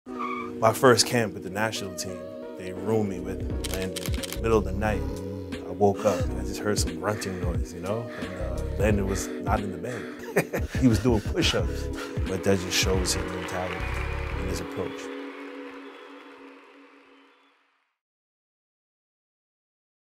WATCH: Edson Buddle Rooms with Landon for First National Team Camp
We get to know a bit about Landon Donovan's worth ethic with a trip down memory lane with former Galaxy and USMNT teammate Edson Buddle.